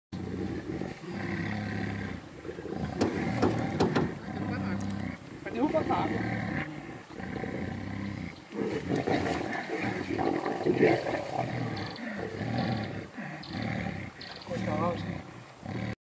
jaguar photography with Pantanal Safaris
Photograph jaguars up close.
Join us on our pantanal jaguar photography tours. We offer the most experienced local guides, the best spots for jaguar photography in the Pantanal. Read more on our tours: